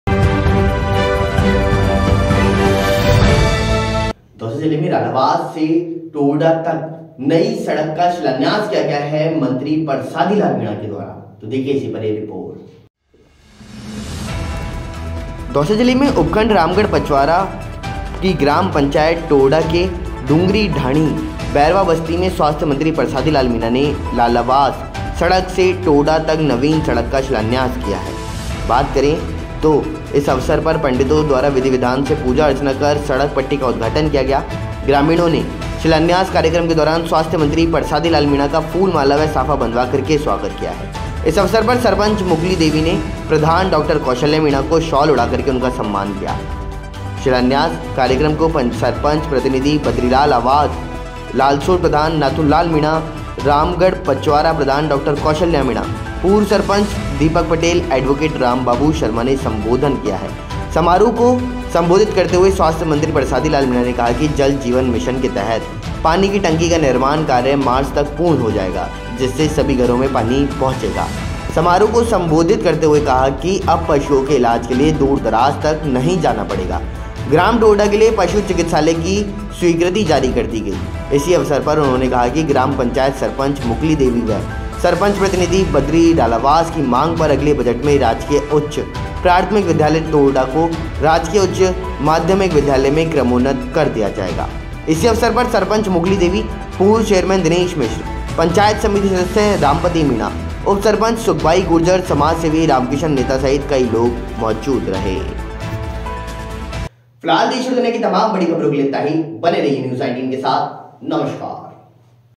स्वास्थ्य मंत्री ने रालावास सड़क से टोरडा तक नवीन सड़क का किया शिलान्यास
स्वास्थ्य मंत्री ने रालावास सड़क से टोरडा तक नवीन सड़क का किया शिलान्यास Health Minister lays foundation stone of new road from Ralavas road to Torda